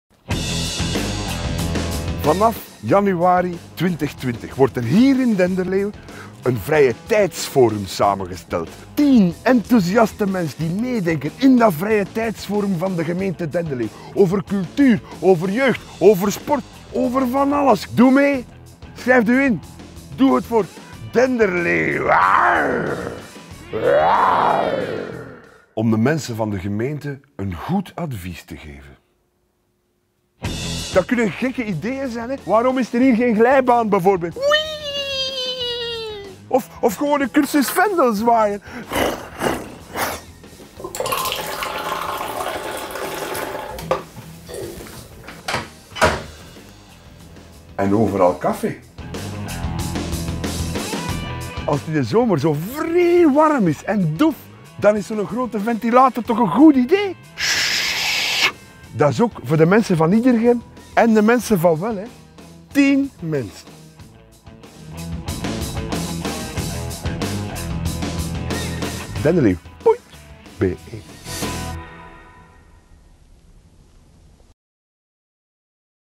Stel je kandidaat voor het nieuwe vrijetijdsforum!
Wij zijn op zoek naar inwoners met een passie voor sport, cultuur, jeugd, senioren of erfgoed. Wil je mee denken over vrije tijd in onze gemeente? Stel je dan nu kandidaat voor het gloednieuwe vrijetijdsforum!